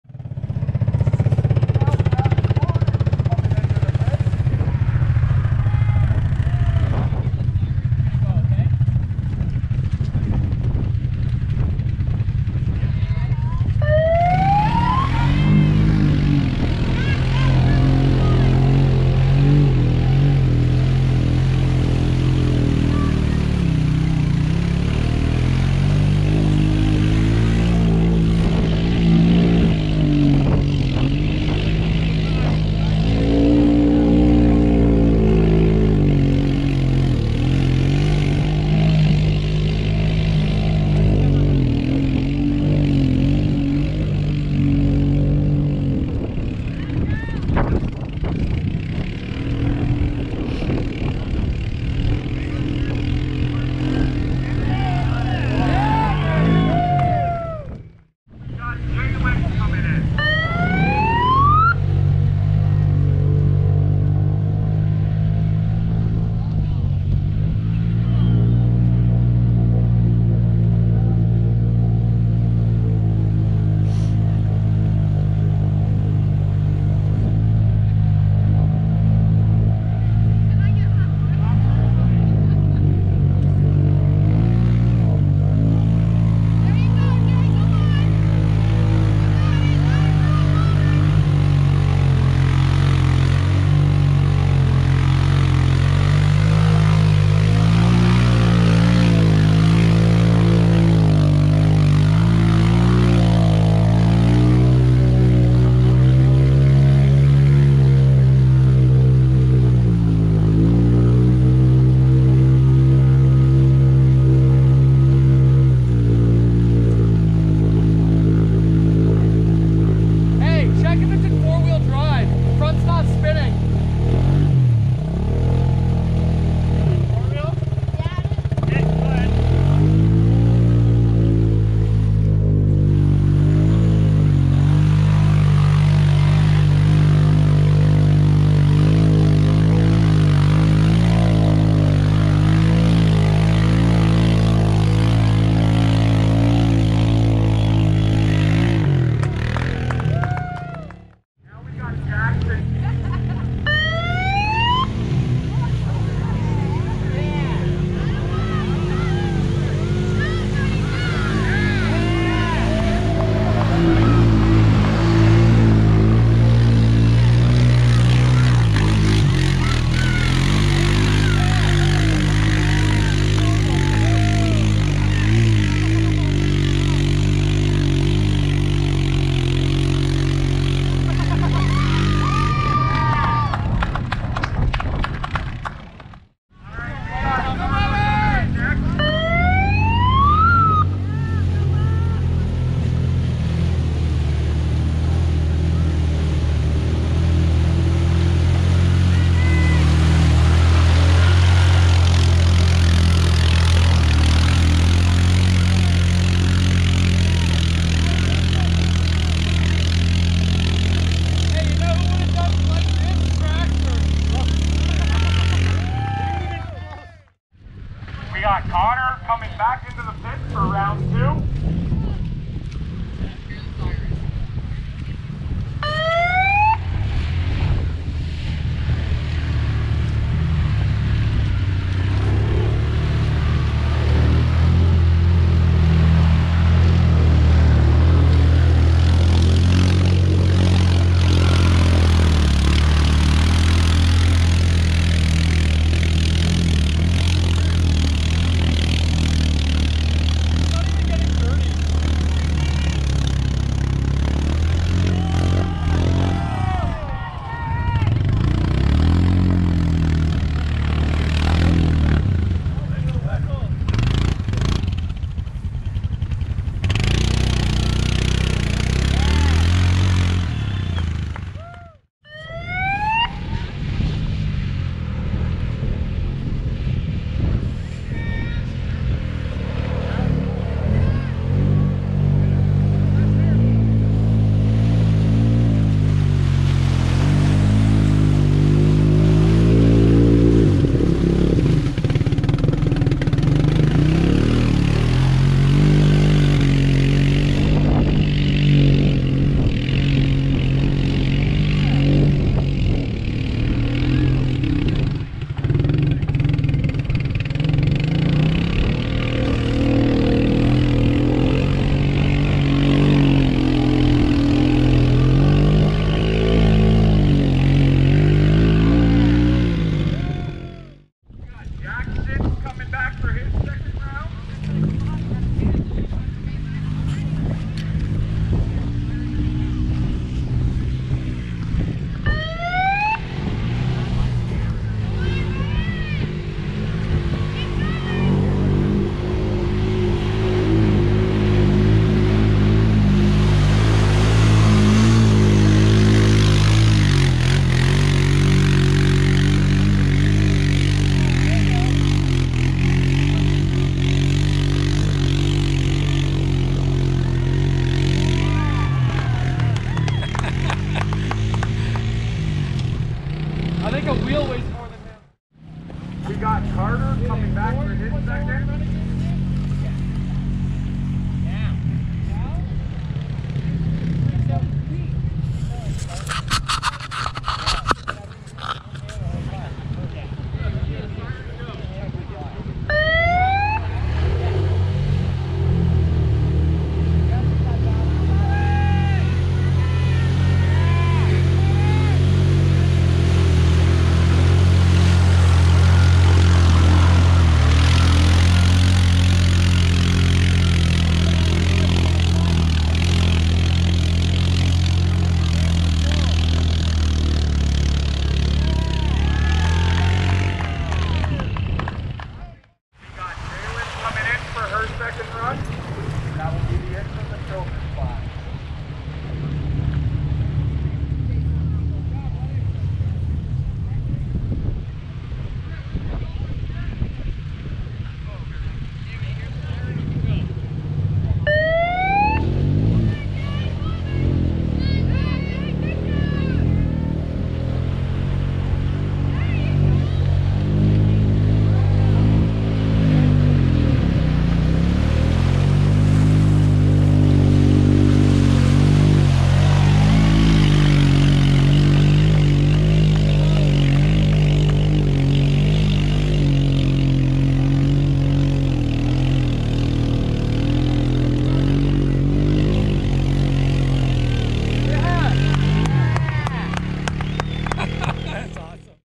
Family playing in the real mud AWESOME!!!!
Niagara Regional Exhibition Fairground in Welland, Ontario. This is the children's event and they stole show, by hit the mud drag as best as they could and showing us how it done!